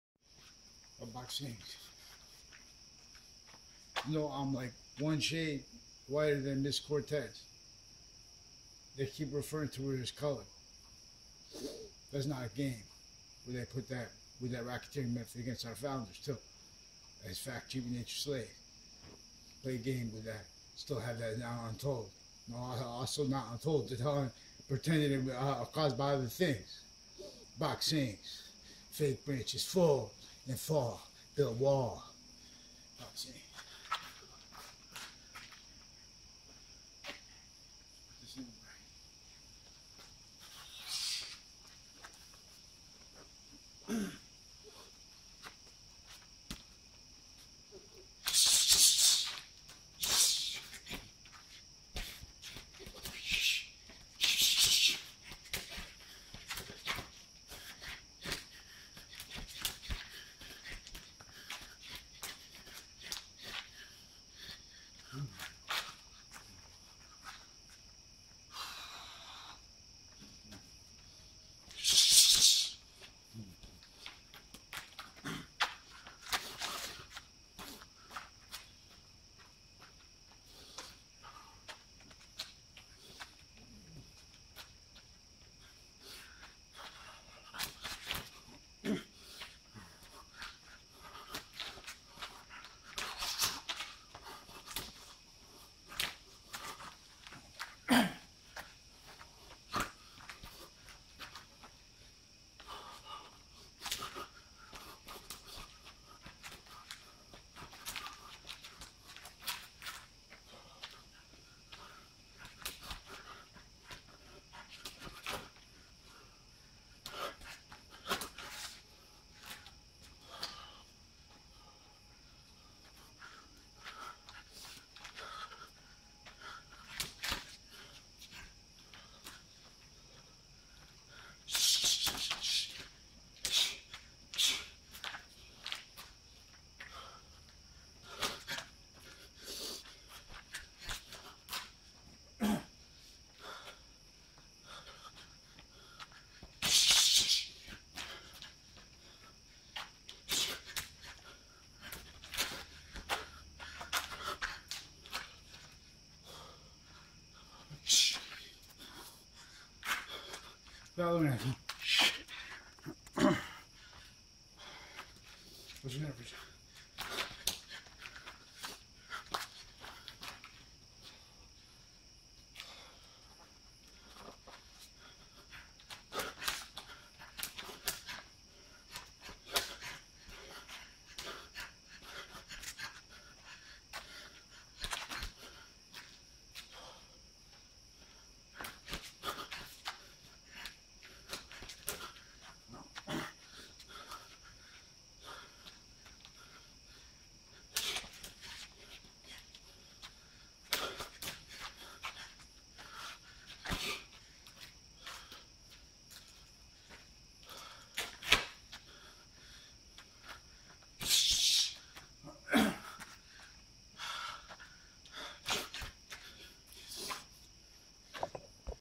Box Sings Tonight for Sports
"Box Sings Tonight for Sports," is a shadow boxing video, by Anthony Wall, A.K.A. Romulus.  Shadow boxing is an awesome exercise.  Shadow boxing is value often missed by women.  Wearing boxing gloves while shadow boxing makes a huge difference.  Shadow boxing is also great for women of sports, like running, and/or for any sport, basically.